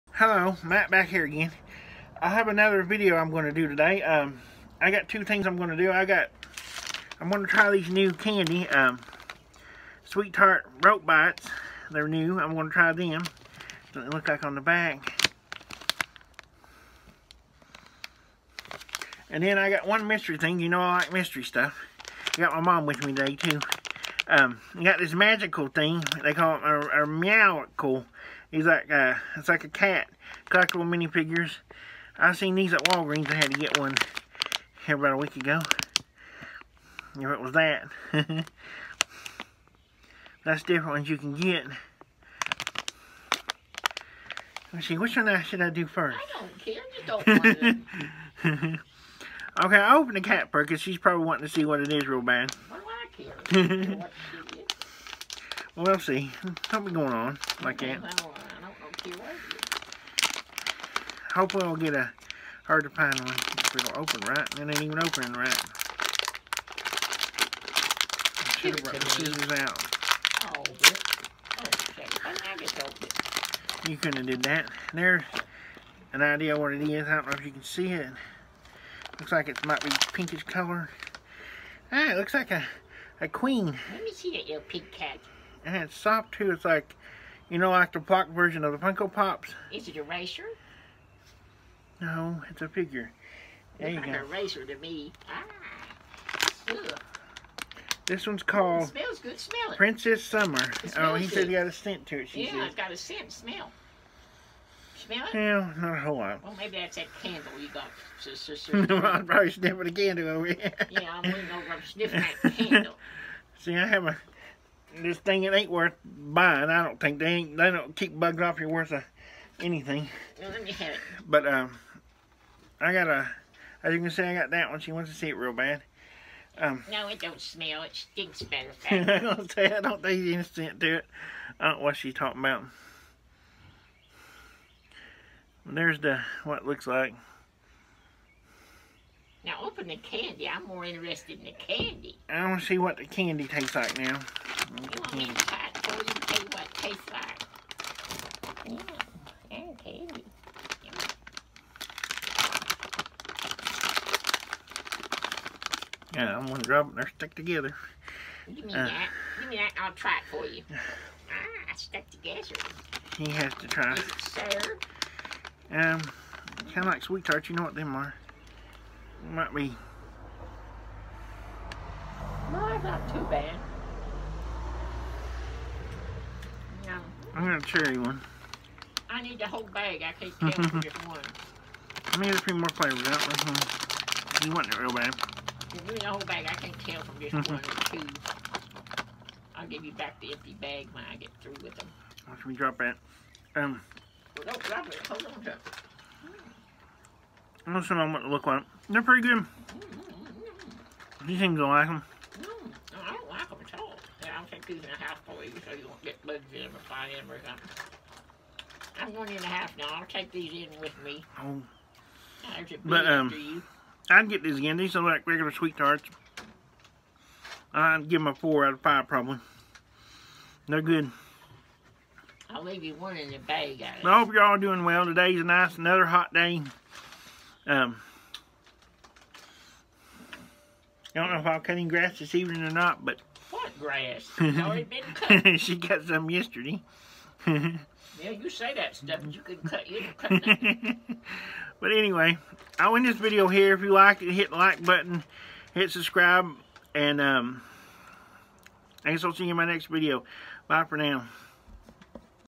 Taste Test - Sweetheart Rope Bites & Opening a Meowgical Collectible Mystery Mini Figure -vlog
Today I taste test new Sweetart Rope Bites & opening a Meowgical Collectible  mystery cat Figure.